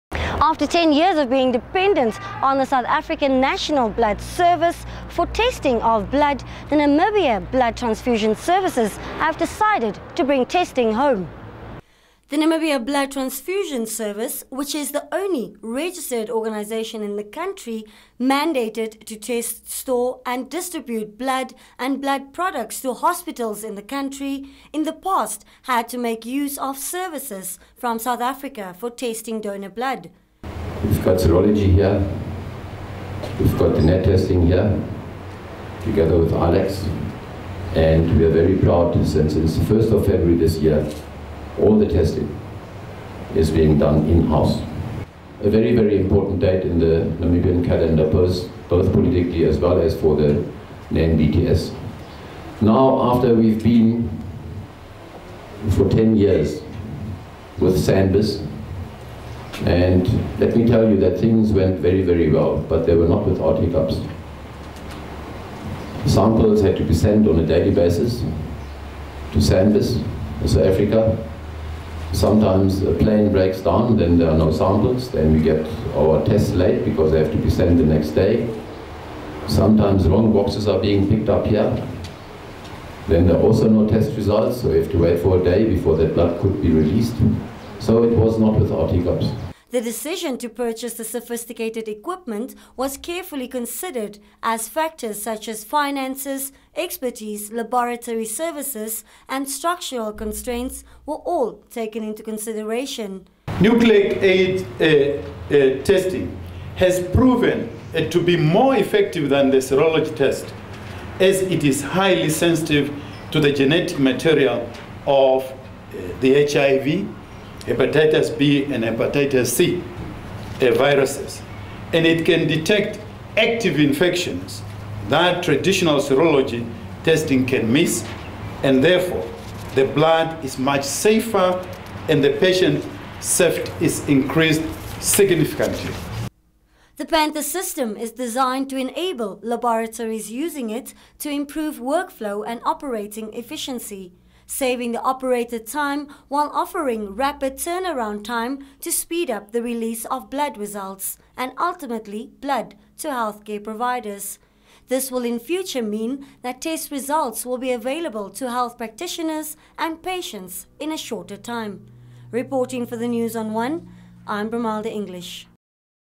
Namibia to do it's own blood safety testing for HIV and Hepatitis
Namibia today became the second country in Africa after South Africa, able to do a hundred percent individual blood donor testing.
Blood safety testing for HIV 1 and 2 Hepatitis B and C was until this year done in South African Laboratories.
Namibia through the Namibia Blood Transfusion Service's attainment of state of the art machines are now able to do all testing locally.